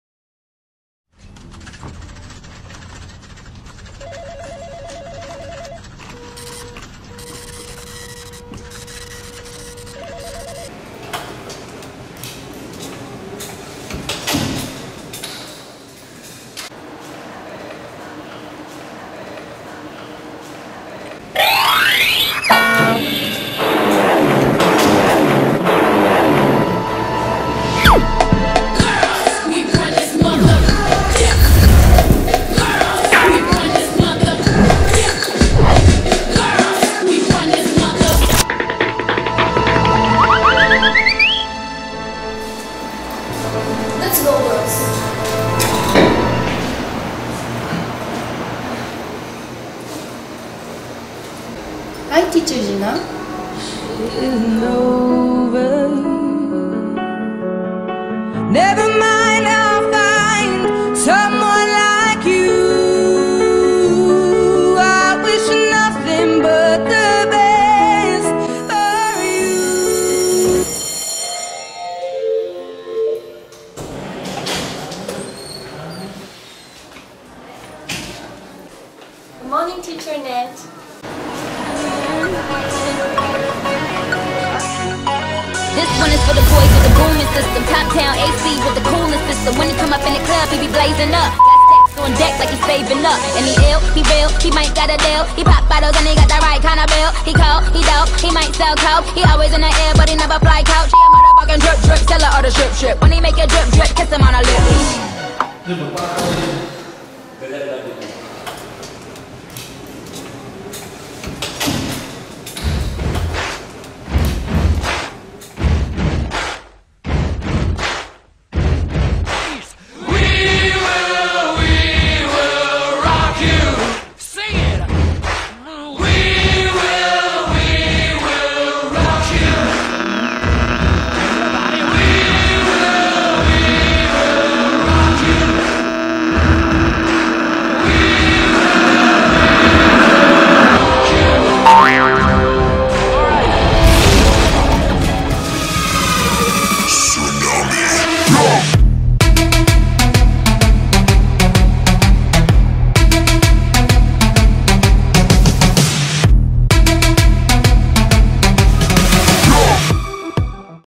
PHILINTER GOT TALENT LIP SYNC AD JUNE 2014 / フィリンタ
Do you know how to sing without uttering a sound? Join the LIP sync competition at the only school that provides fun and learning at the same time, Philinter Education Center. 'Expose yourself to learn English the Right Way'. See you on June 27, 2014 Live in Cebu, Philippines, at 1:30 pm.